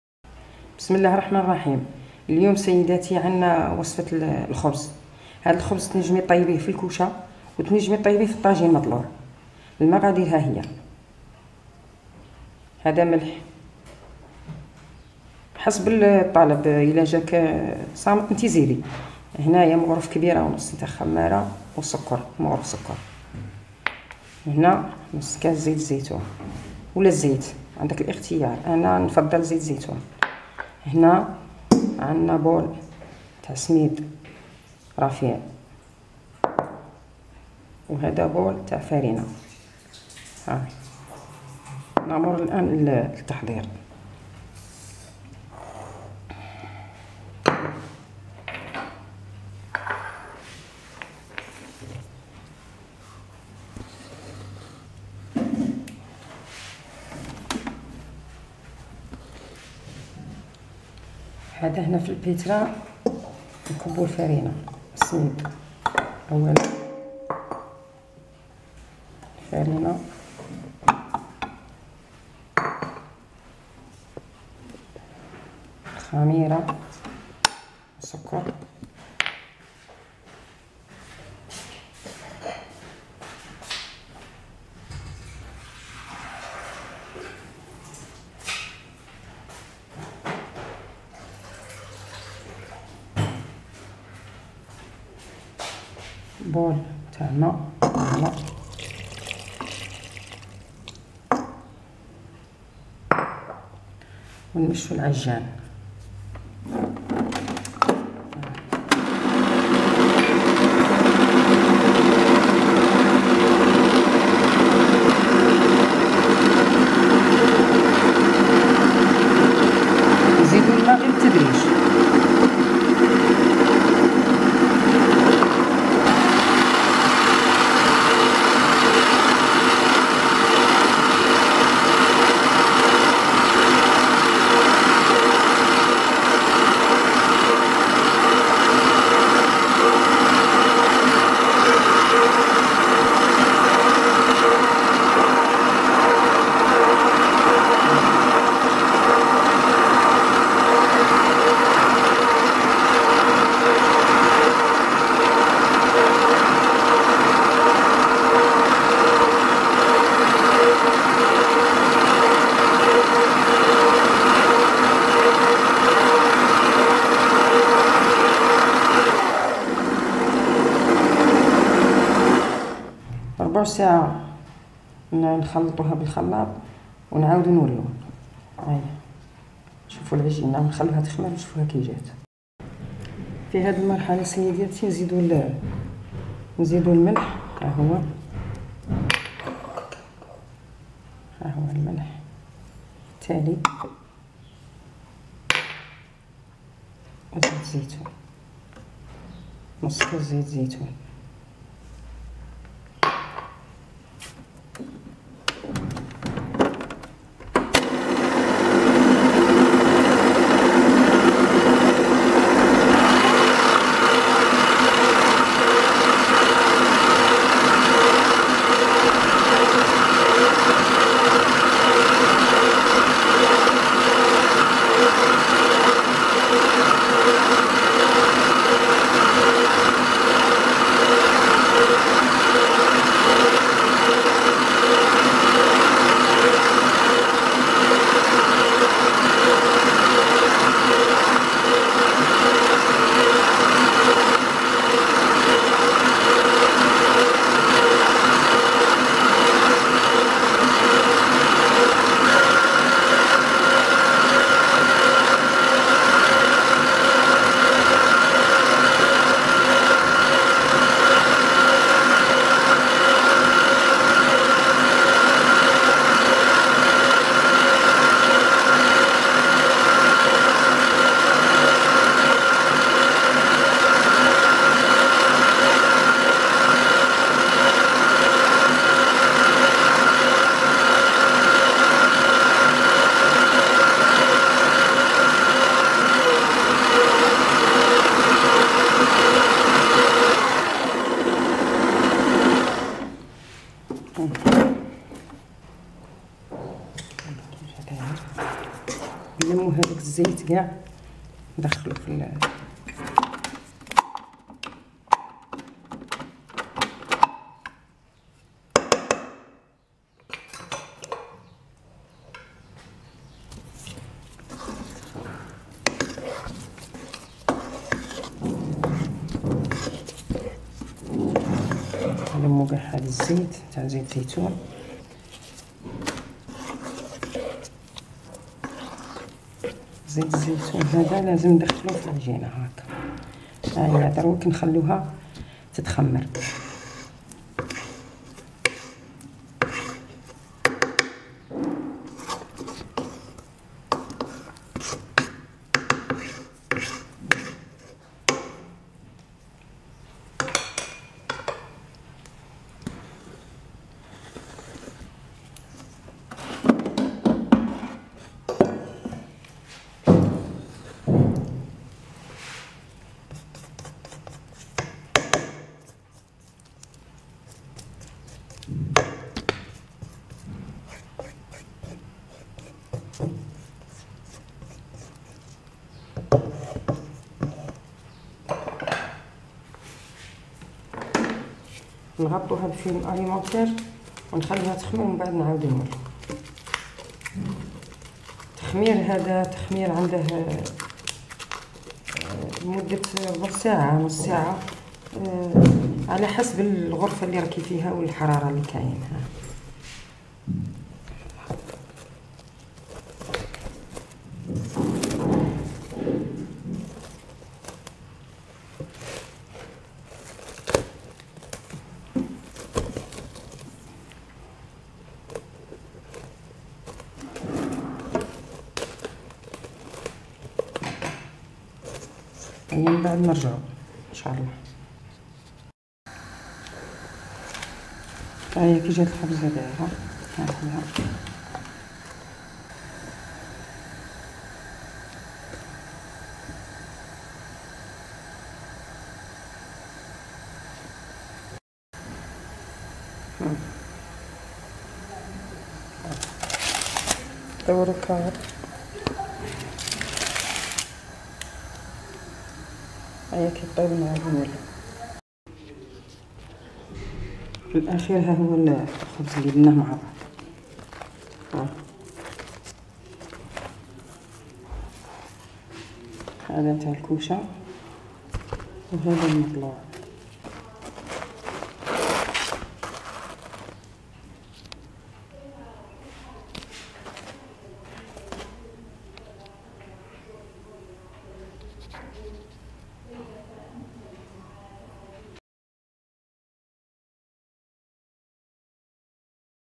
خبز او اغروم الصحي
خبز  خفيف و طيب المذاق ياكل مع الحساء او الشوربة.